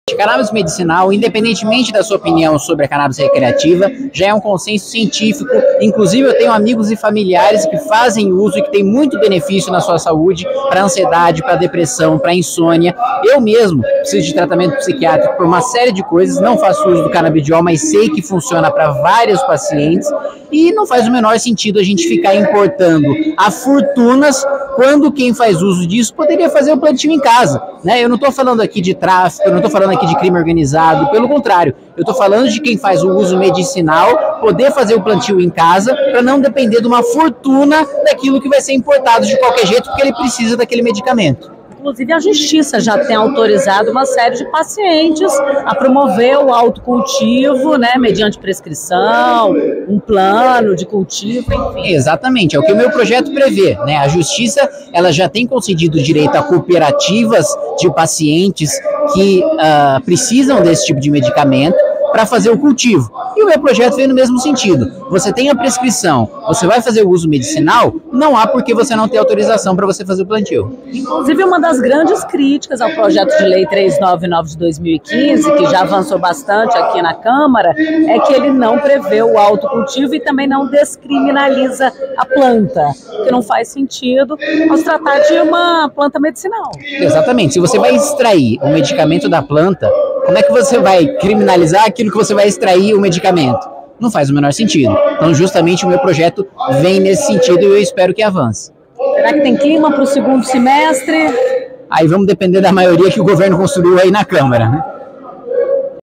A Cannabis no Cogresso entrevista o deputado federal Kim Kataguiri (União/SP).
Recentemente, o parlamentar apresentou um projeto de lei para autorizar o autocultivo da Cannabis por pacientes que têm prescrição médica.
Quer entender melhor? A reportagem completa no Portal Cannabis & Saúde.